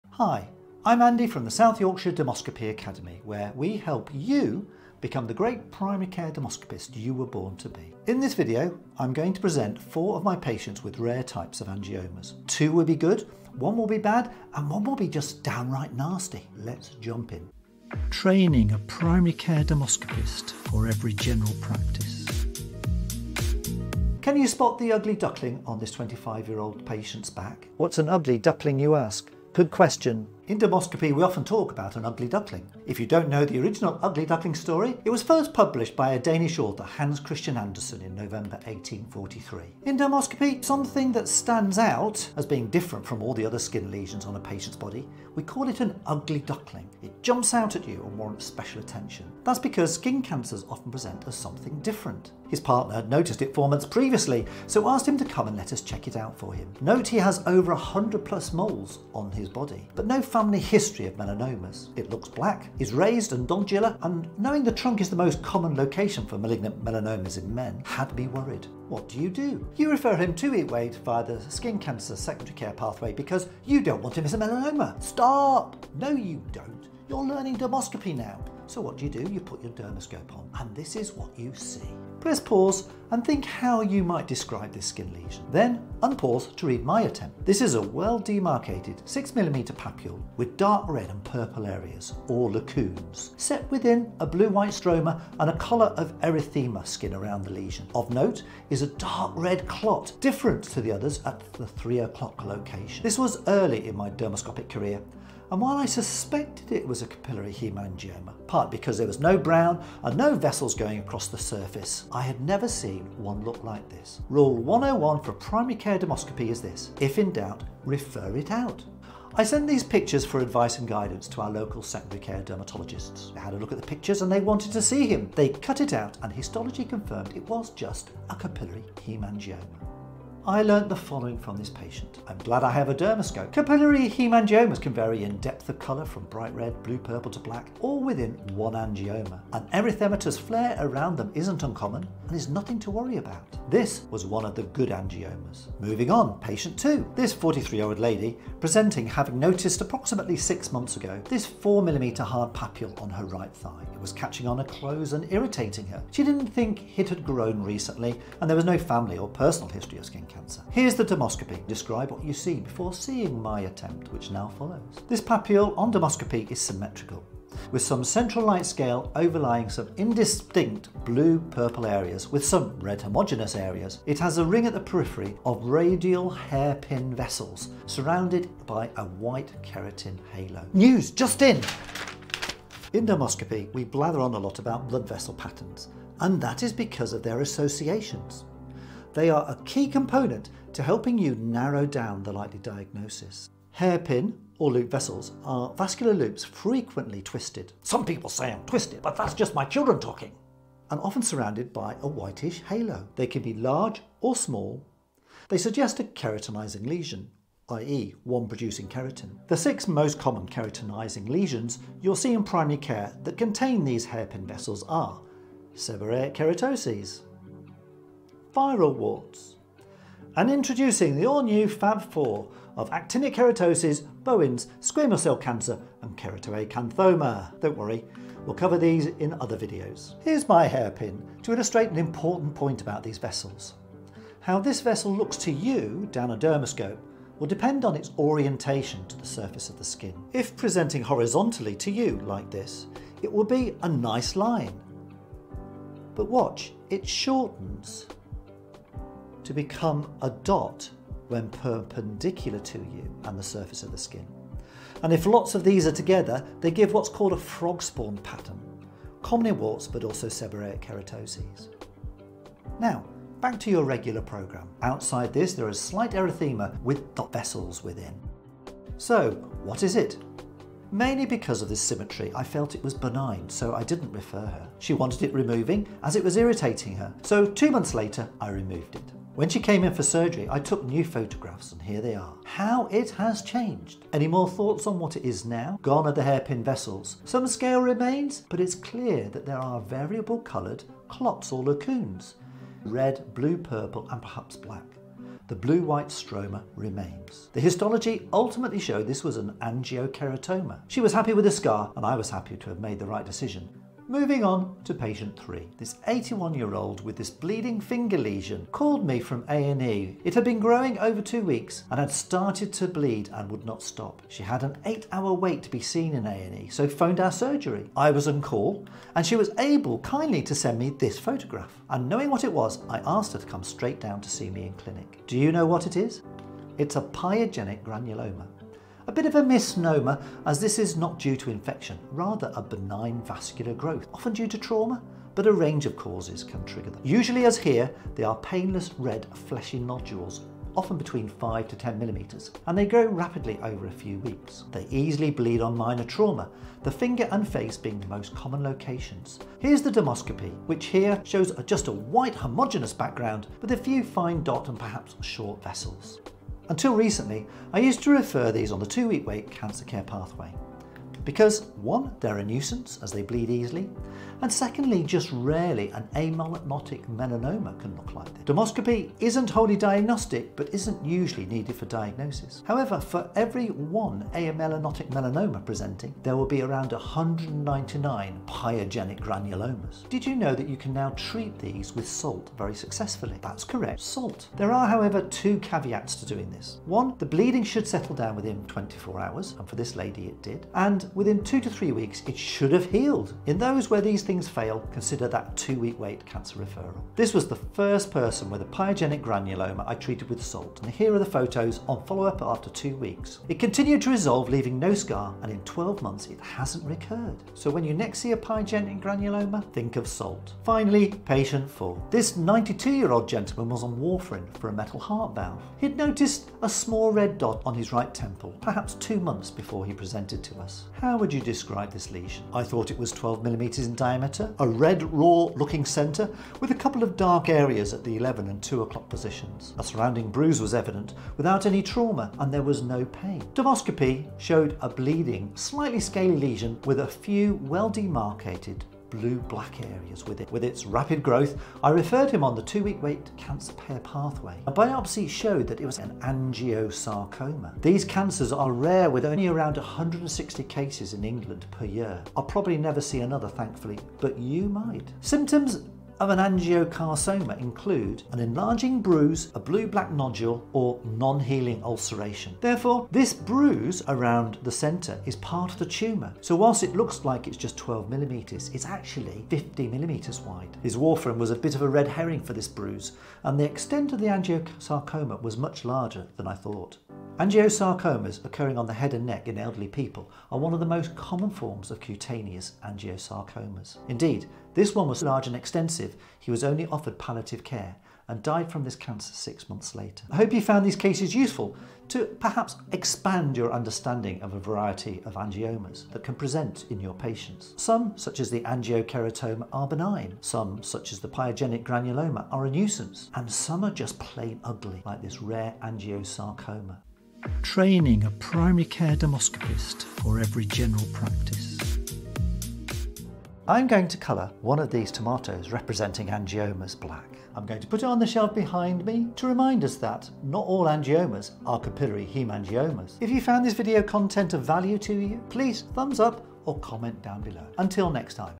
The dermoscopy of Angiokeratomas, Angiosarcomas, and salt for Pyogenic Granuloma!
00:25  The 'Ugly Duckling' [The Good]
01:53  Rule 1 for Primary Care Dermoscopy
02:26  Angiokeratoma [The Good]
03:00  Understanding 'Hair Pin' vessels
05:03  Pyogenic Granuloma [Bad]
06:20 Treating with SALT
06:52  Angiosarcoma [The Ugly]